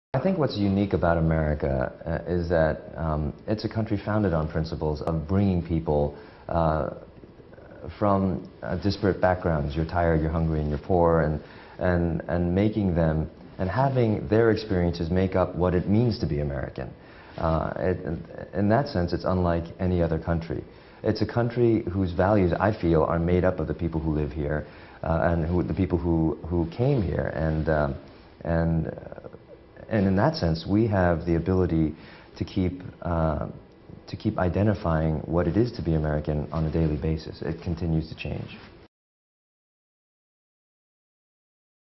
Actor Daniel Dae Kim on What it Means to be an American
Korean-American actor Daniel Dae Kim (Lost, Hawaiii Five-0) discusses the unique and diverse American identity.
This video was produced by the Bureau of International Information Programs in May 2011.

Transcript:
I think what's unique about America is that
it's a country founded on principles of bringing people together from disparate backgrounds —
your tired, your hungry and your poor — and
making them — and having their experiences make up what it means to be American.

In that sense it's unlike any other country.
It's a country whose values I feel are made up of the people who live here,and the people who came here. And in that sense we have the ability to keep — to keep
identifying what it is to be American on a daily basis; it
continues to change.